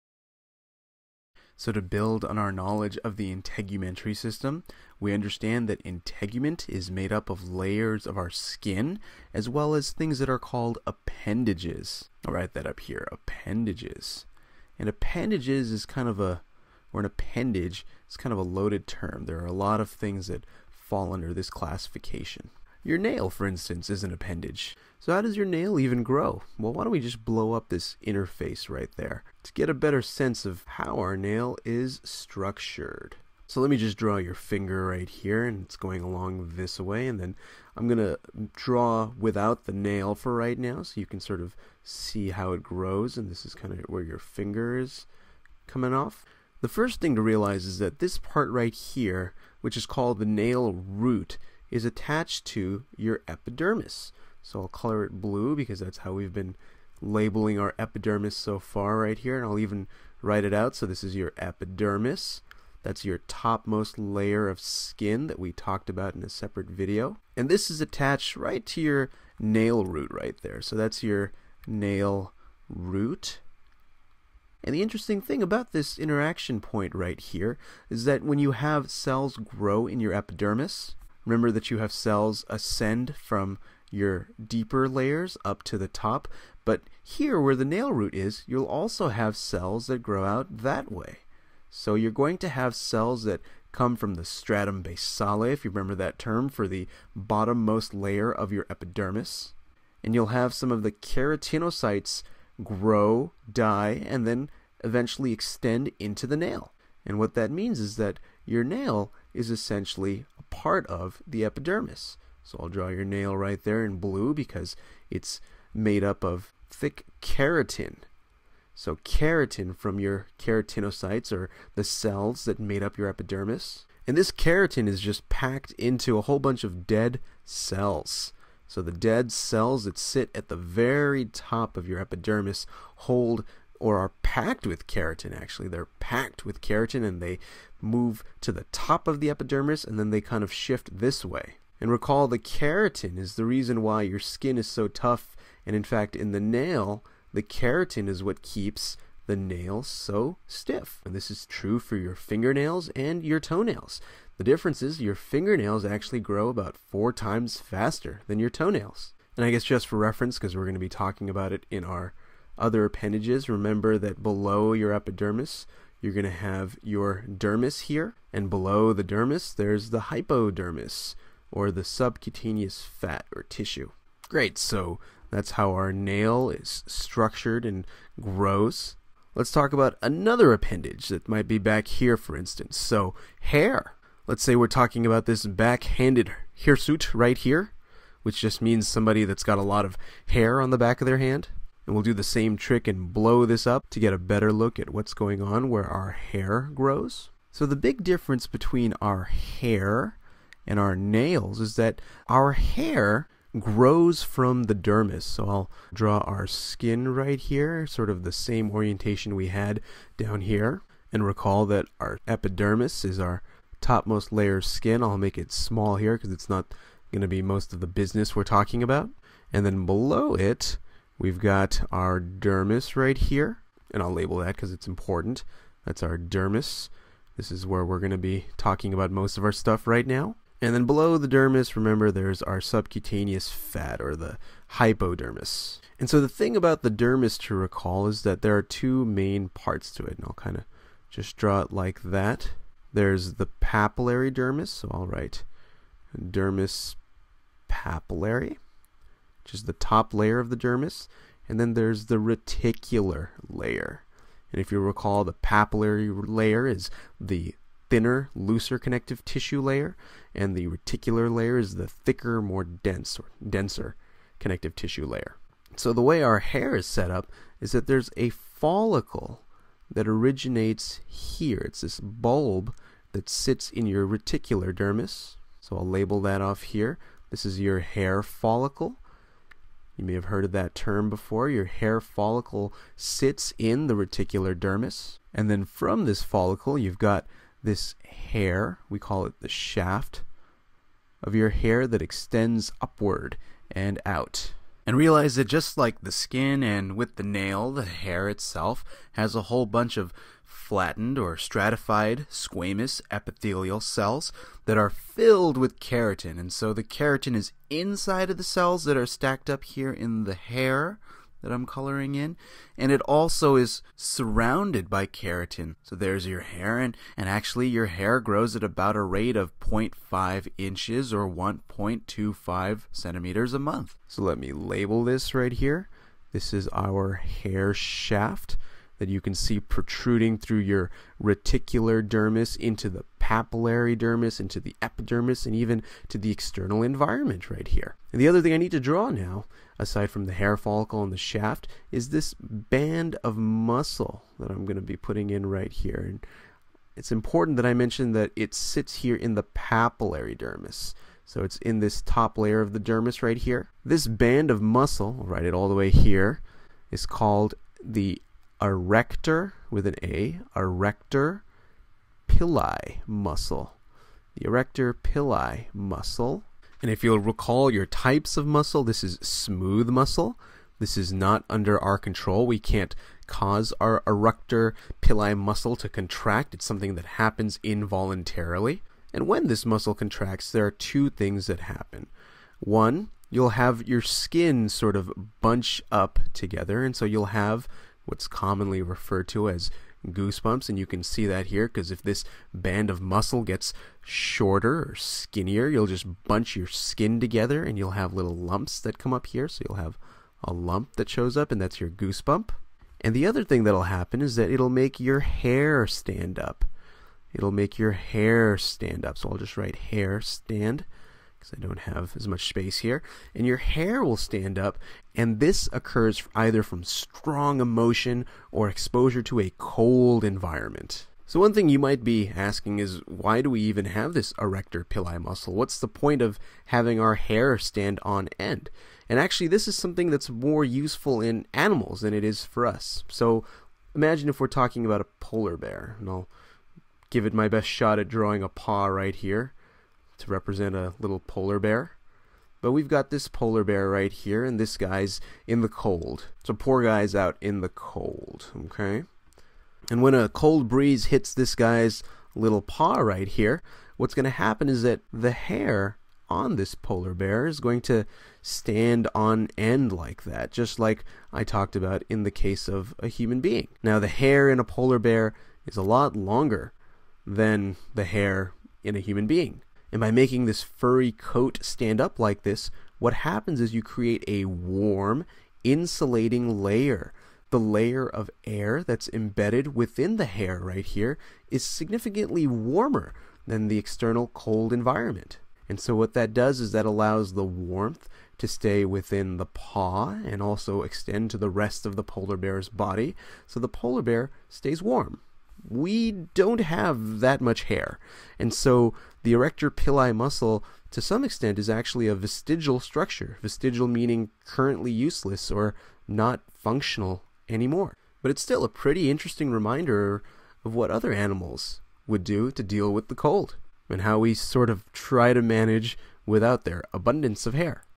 Where do our nails and hair come from? | Integumentary system physiology | NCLEX-RN | Khan Academy
Created by Raja Narayan.